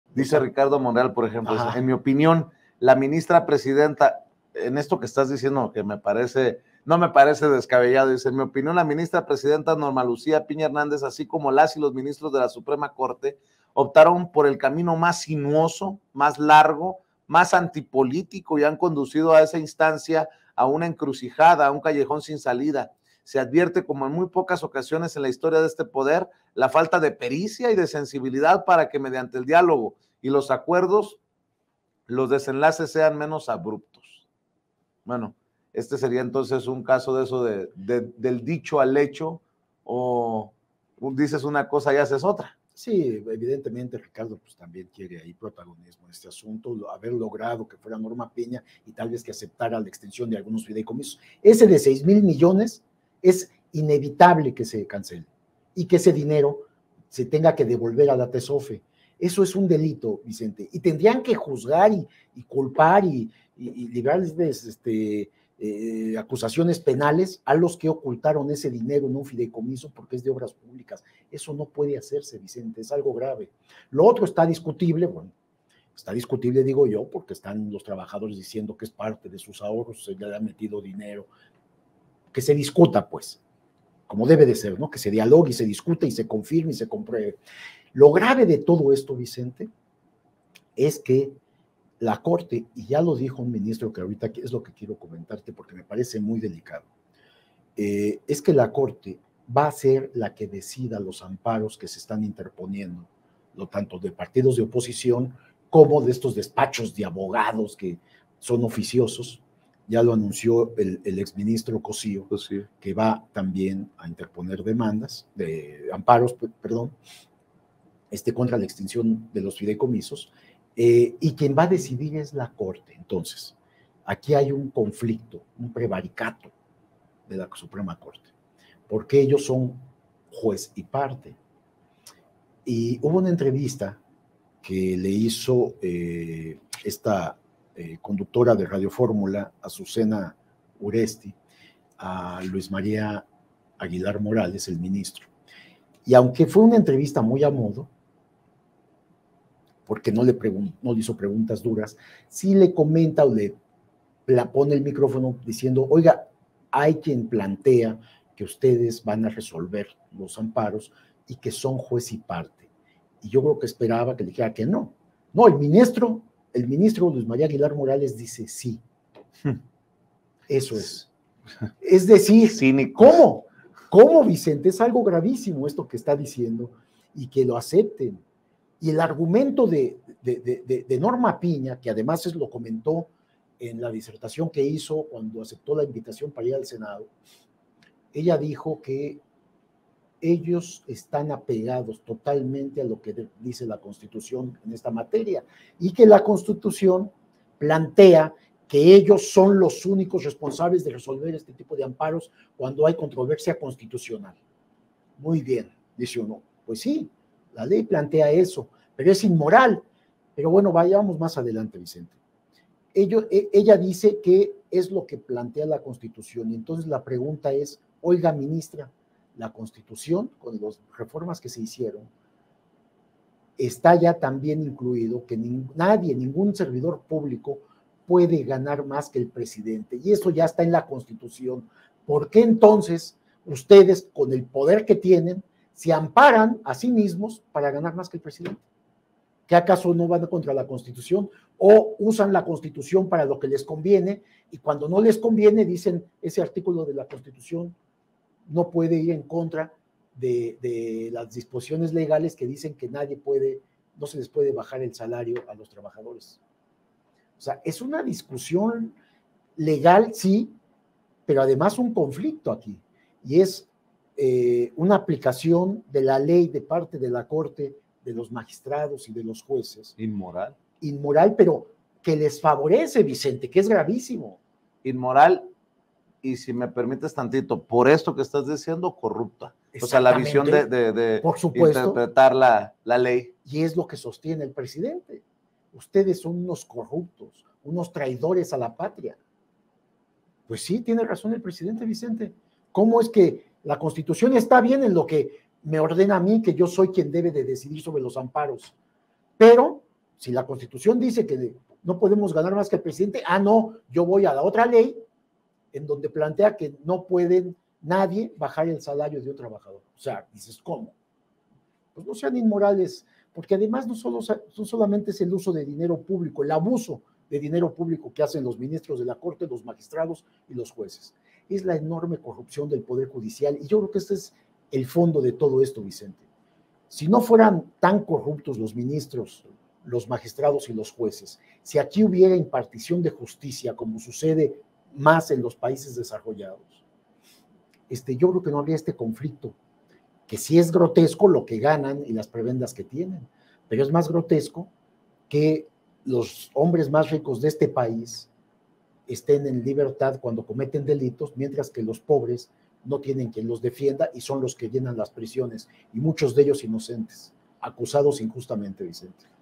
LOS MINISTROS Y MINISTRAS OPTARON POR UN CAMINO ANTIPOLÍTICO.
Inevitable que se devuelva un fideicomiso de 6mil millones, es un delito y debe haber consecuencias. Ningún servidor público puede ganar más que el Presidente.

Miguel Badillo: Director de Contralínea